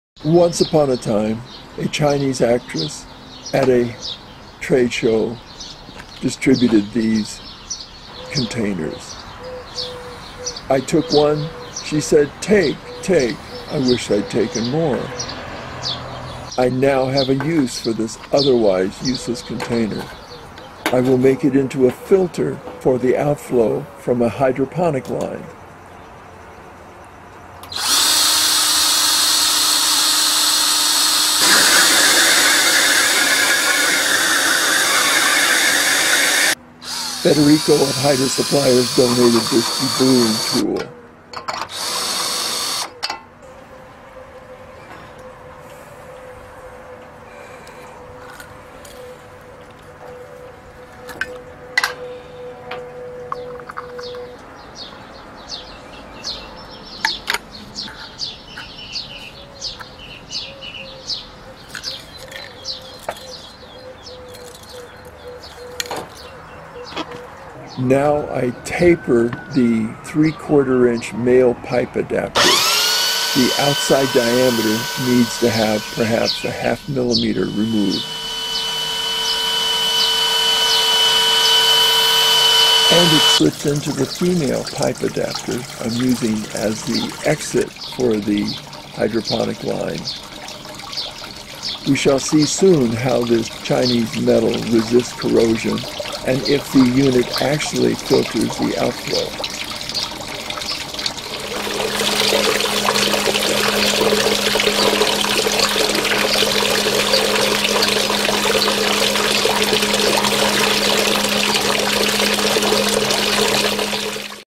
A Gift from a Chinese actress -- I Attempted To Make A Hydroponic Filter
At a cinema trade show, a lovely Chinese actress distributed these film containers filled with candies and business cards.

China Guangdong Huazhijie Film & TV Broadcasting Company, Ltd.

She held out one to me, then offered me another:  "Take, take."

So lovely, her pale perfect features, the glossy red of her lips, her lustrous dark hair.  So slender, so lovely.

I wanted to take more.

The mesh looked like stainless steel.  I could use the mesh container in my hydroponic system.

A problem with intensive hydroponics.  Debris.

Roots.  Dirt.  Algae scum.  Fragments of broken clay pebbles.  All of the debris accumulates in the system and clogs water jets.

I had the container from years ago.  ( Where is she now?  So lovely, so slender.)  I looked at that mesh, I thought.  Why not try it as a filter?

This only required standard tools, a sander or grinding wheel, and three plumbing fittings -- a 3/4 inch female pipe adapter, a 3/4 inch male pipe adapter, and a 1 inch male pipe adapter.

The deburring tool came from HydroSuppliers.  Donated by Federico.  Excellent tool.  Wish the deburrers came larger.  I'm working with larger fittings and the tool only goes to 1 inch.

The video shows the process.

And what she gave me?  Trash.  Not stainless steel.  It immediately rusted, and as the months passed, rotted.

I finished this upload at 9:30pm.  At dawn tomorrow, I salvage the PVC and trash the filter.  Before the rusted metal falls into the reservoir and threatens the pump.